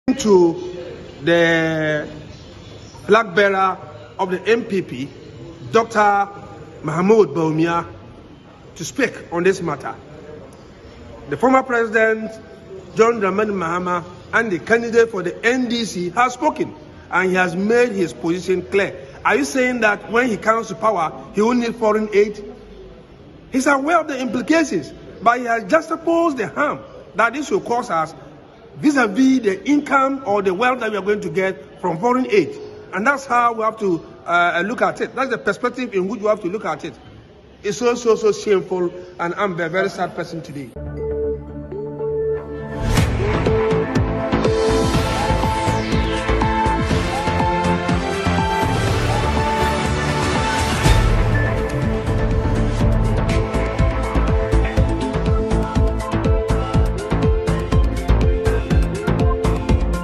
Anti-LGBTQ+ Bill: Dr Bawumia should publicly air his stance on the bill - Yusif Sulemana
Anti-LGBTQ+ Bill: Dr Bawumia should publicly air his stance on the bill - MP, Bole Bambo, Yusif Sulemana.